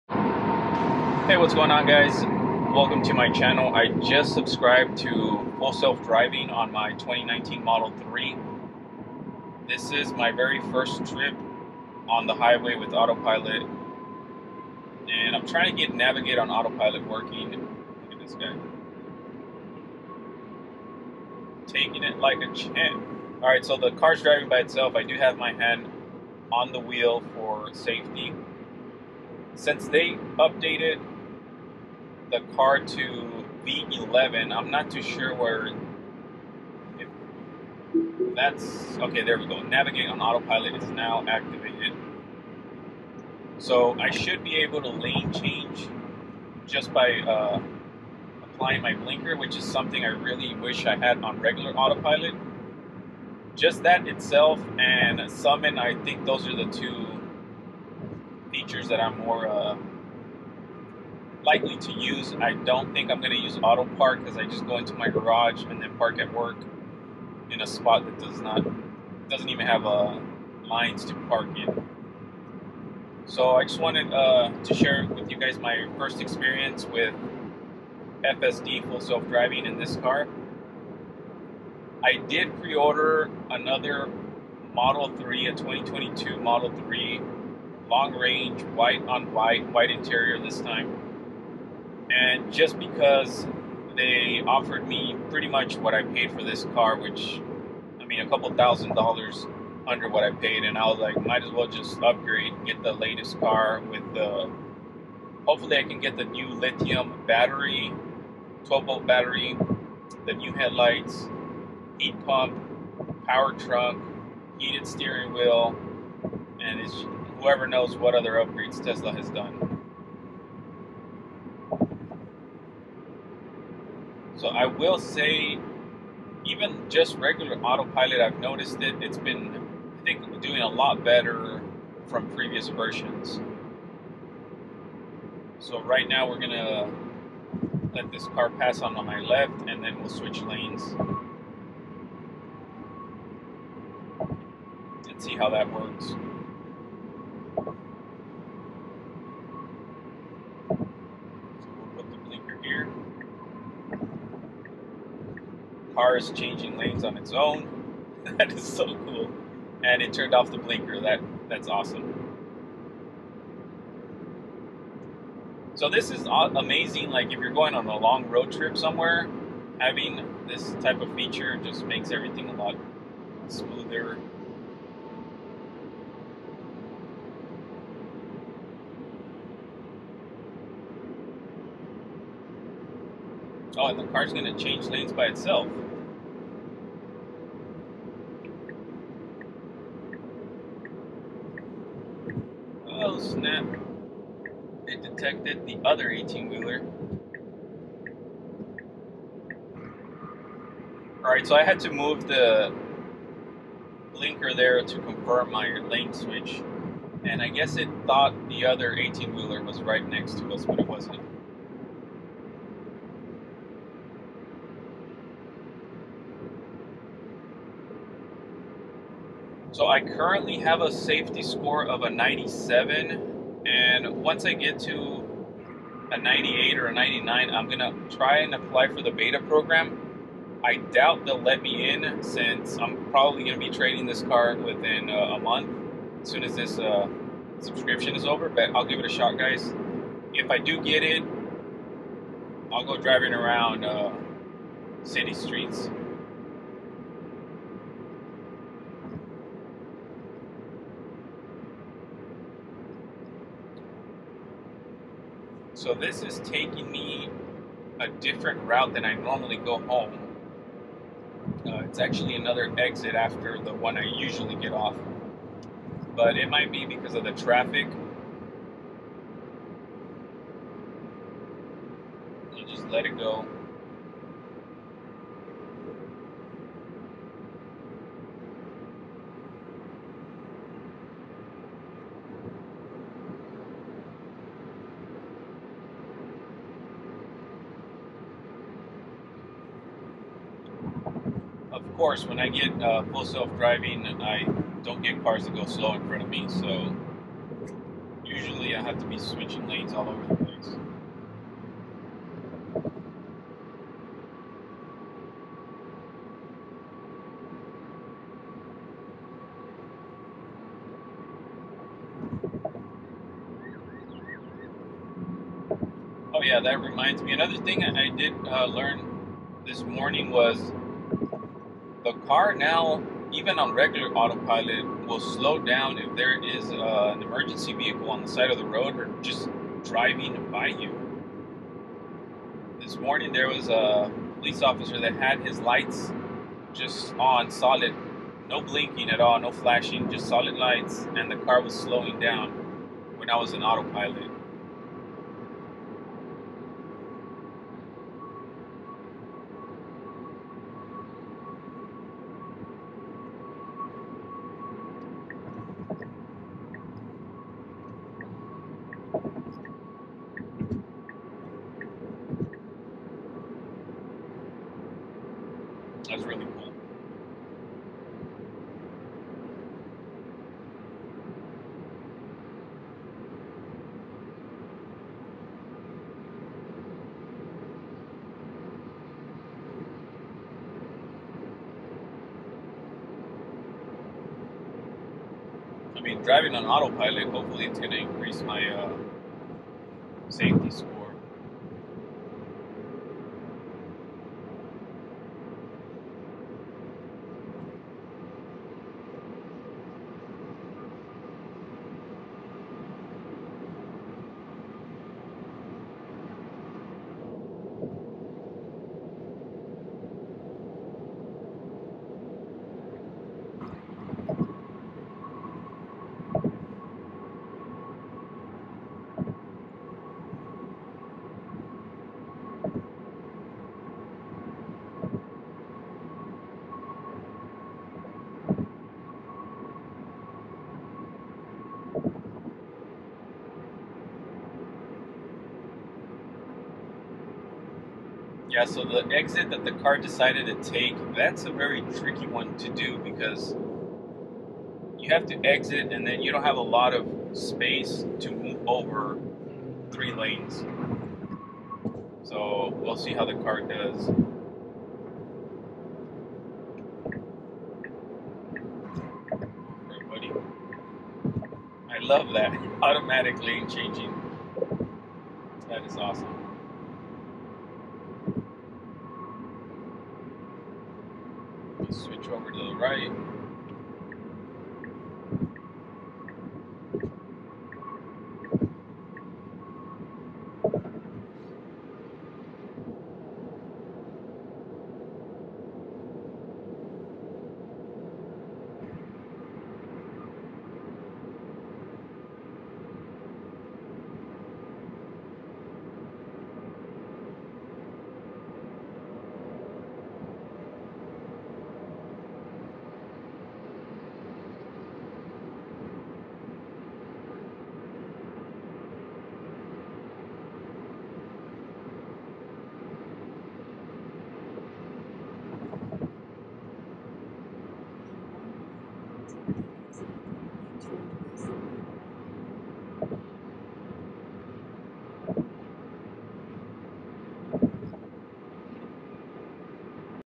My First Drive w/ Tesla FSD Navigate On Autopilot
I wanted to try it out before I bought the software on my new 2022 Model 3. In this video you will see the car do lane changes on its own.